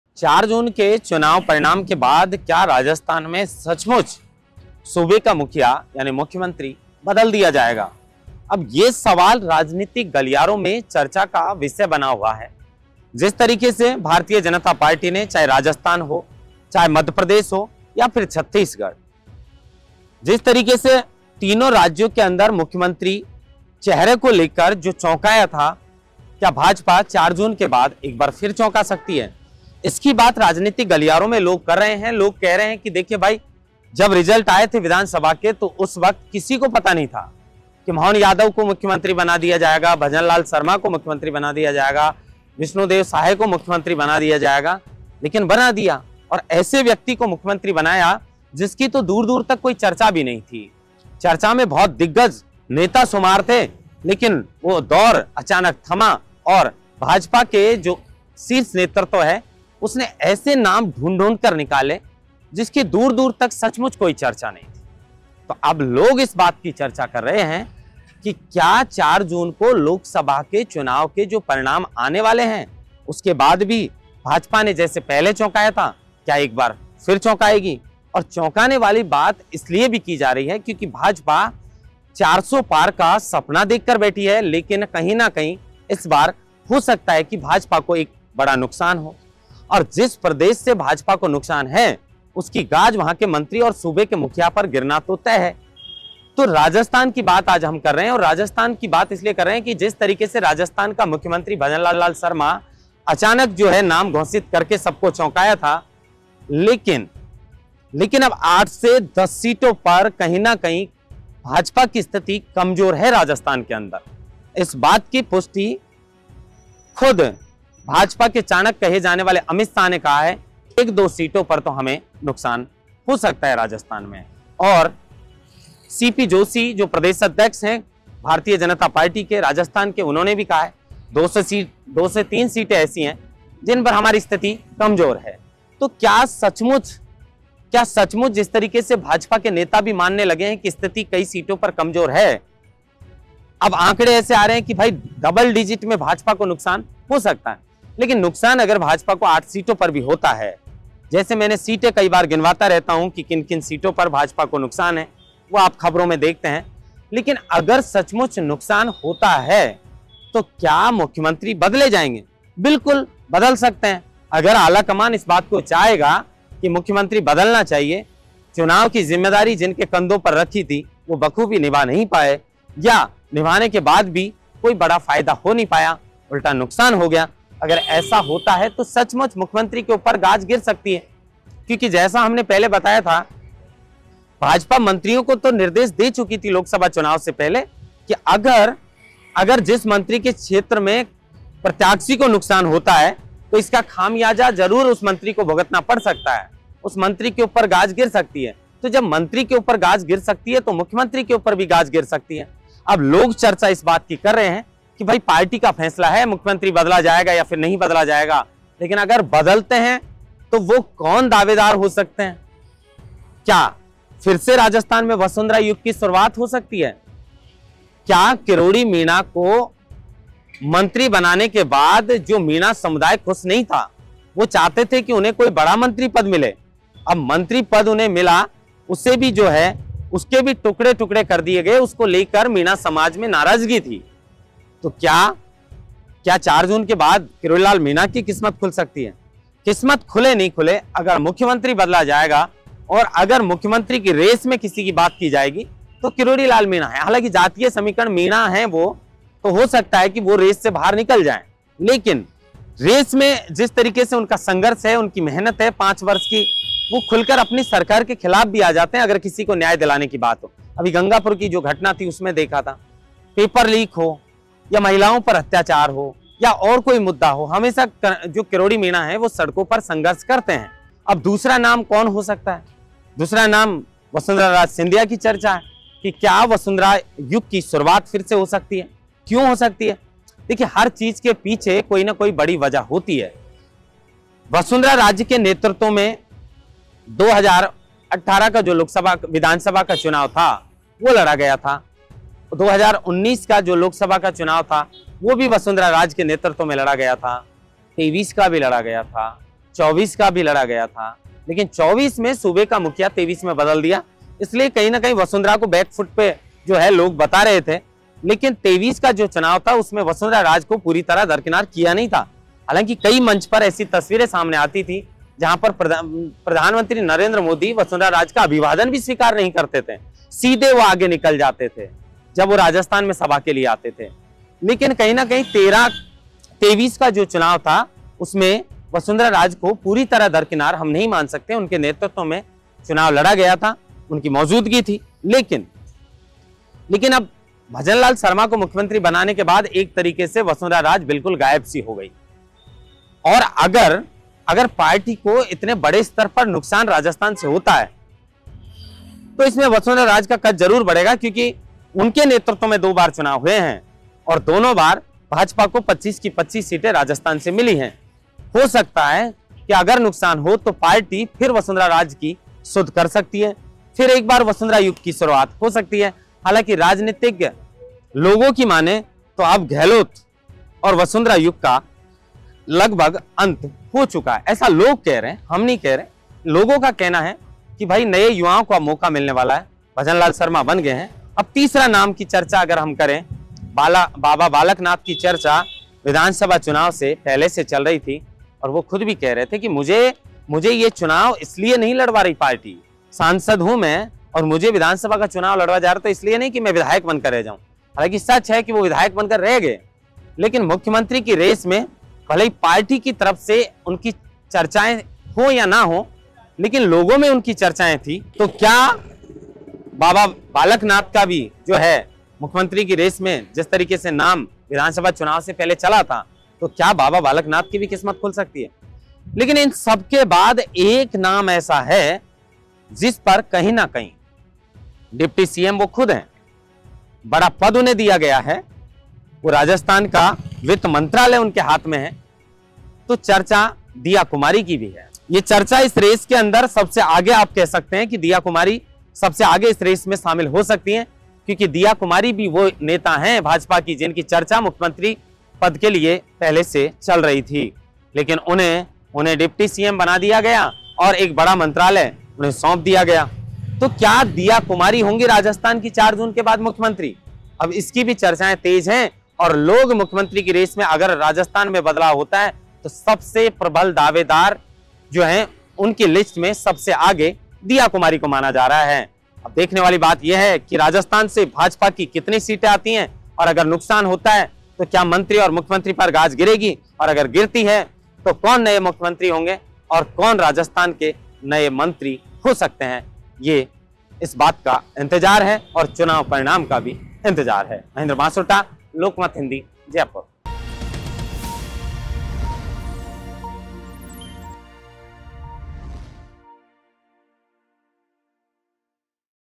RAJASTHAN CM-4 जून बाद कौन हो सकता है राजस्थान का मुख्यमंत्री, भजनलाल शर्मा की कुर्सी को ख़तरा?
राजस्थान में 4 जून  चुनाव परिणाम के बाद मुख्यमंत्री बदला जाएगा इसको लेकर राजनैतिक गलियारों में चर्चाएँ तेज होने लगी है अब लोग यही बात कर रहे हैं कि अगर राजस्थान में सीटों का बड़ा नुक़सान होता है और अगर मुख्यमंत्री बदला जाएगा तो कौन हो सकता है नए मुख्यमंत्री पद का दावेदार इस रेस की अगर हम बात करें तो विधानसभा चुनाव में भी कई ऐसे नाम थे जिन की चर्चाएँ हर कोई कर रहा था इस रेस के किरोड़ी लाल मीणा, दिया कुमारी,बाबा बालक नाथ,वसुंधरा राजे सहित कई दिग्गज इस रेस में शामिल थे लेकिन भाजपा ने छत्तीसगढ़ मध्य प्रदेश के बाद राजस्थान में भी चौकाया और अचानक 1 ऐसा नाम मुख्यमंत्री नाम घोषित किया जिसको लेकर हर कोई चौक गया था उस समय राजस्थान की कमान भजनलाल शर्मा को सौंपी गई लेकिन अब 1 बार फिर लोग इसकी चर्चा कर रहे हैं कि राजस्थान में 4 जून के बाद मुख्यमंत्री बदला जा सकता है